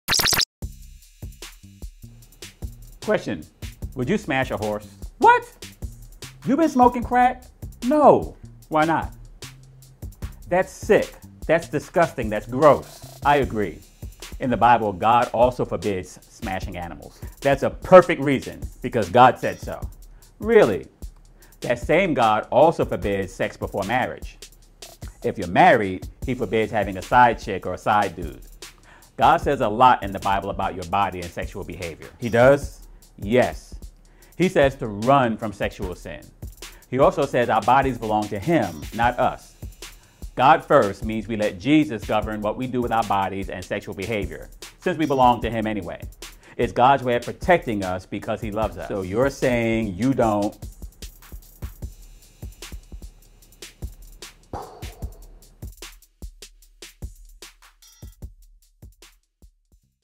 Would you smash a horse? Is GODFirst With Your Body?
Jesus says to RUN from sexual sin [1 Cor. 6:9 NLT]. He also says our bodies belong to Him, not us. GODFirst means we let Jesus govern what we do with our bodies and sexual behavior, since we belong to Him anyway. It’s God’s way of protecting us because He loves us.